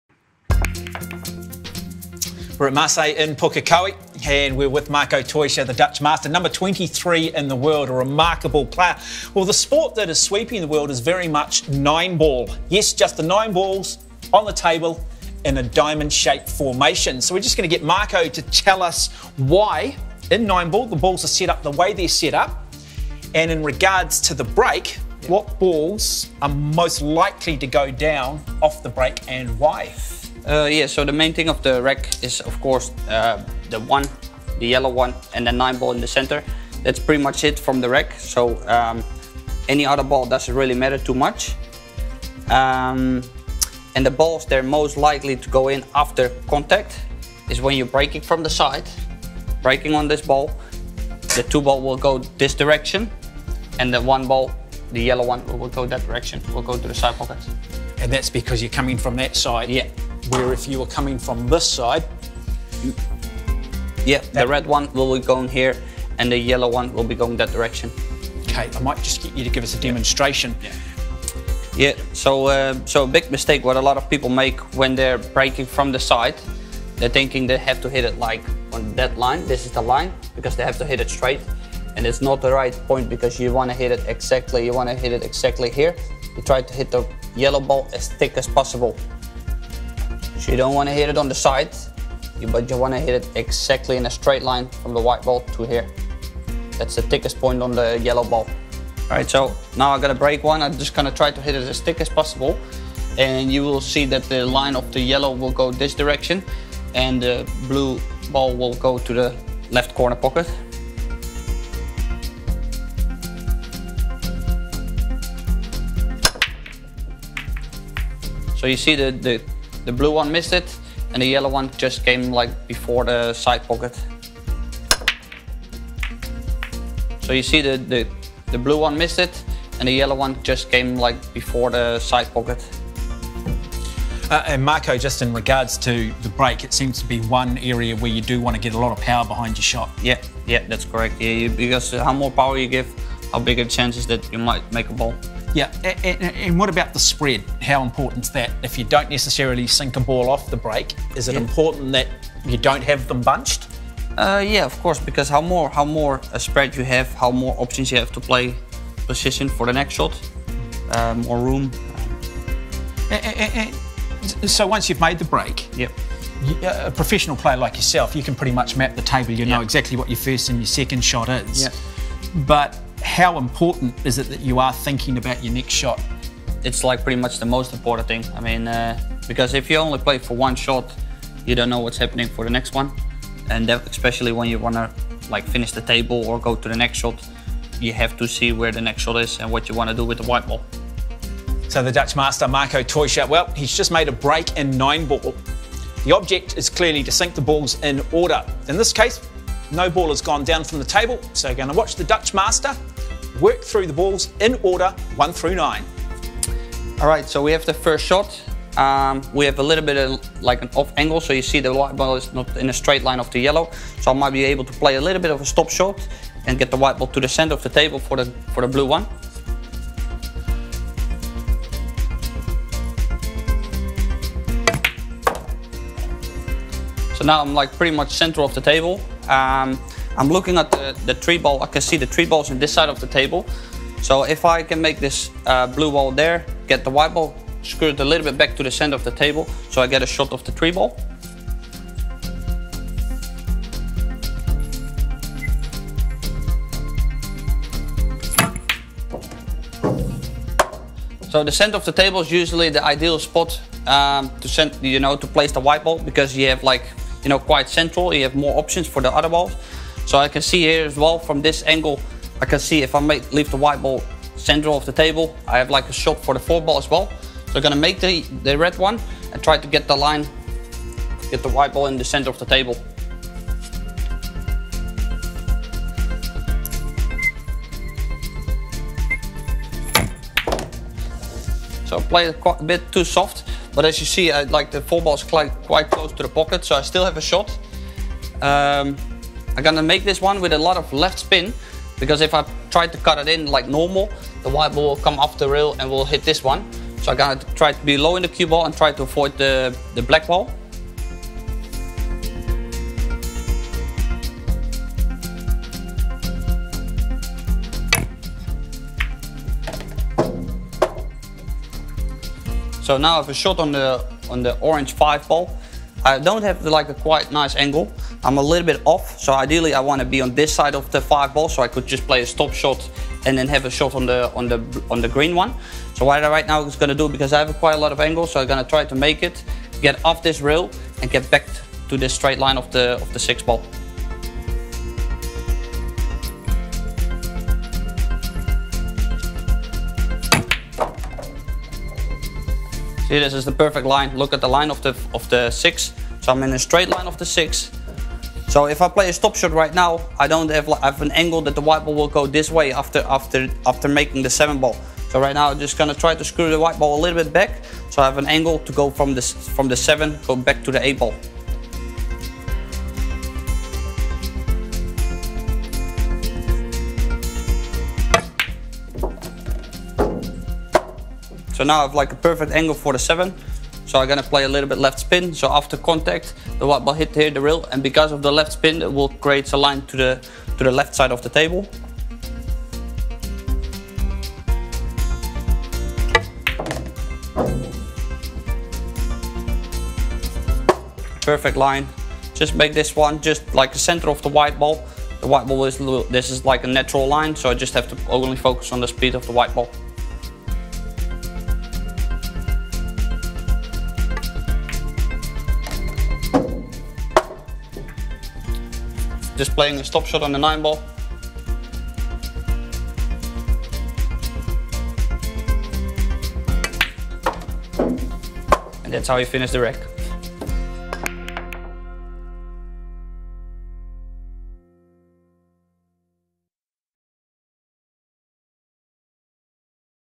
Cue Sports Tutorial 9 - Nine Ball
Sports broadcaster Mark Watson presents a series on the basic fundamentals of playing cue sports. Mark is joined by the 2019 US Open 10 Ball Champion, and professional player, Marco Teutscher, to guide the beginner player through the various basic aspects of playing cue sports. In this ninth episode, Mark and Marco introduce the viewer to the basics of the game of Nine Ball.